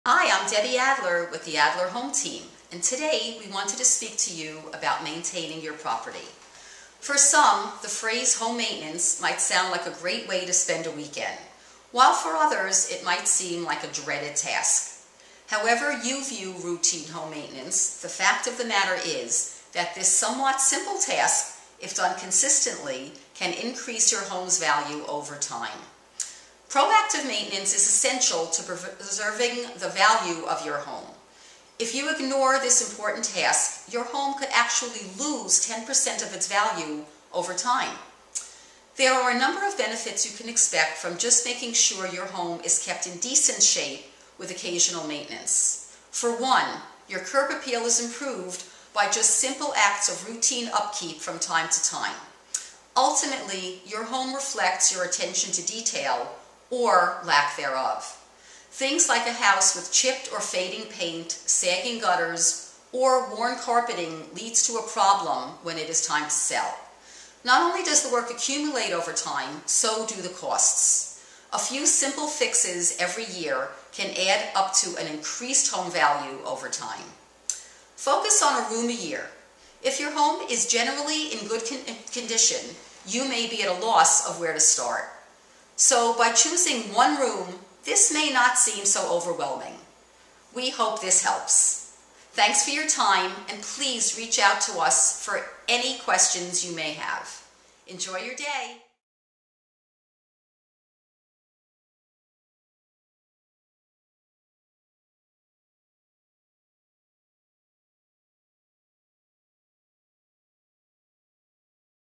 The Adler Home Team - Home Maintenence
Do you perform all of the tasks that need to be done each year to keep your home in great shape? With a hectic schedule and a tight pocket book, it's easy to overlook a few tasks around the house that are just begging to be done. But, what if there were more to home maintenance than just slapping on a new layer of paint and fixing that broken step? Let me show you why simple home maintenance tasks can actually put money in your pocket in the coming years.

The Adler Home Team
Keller Williams Realty
Bergen County, New Jersey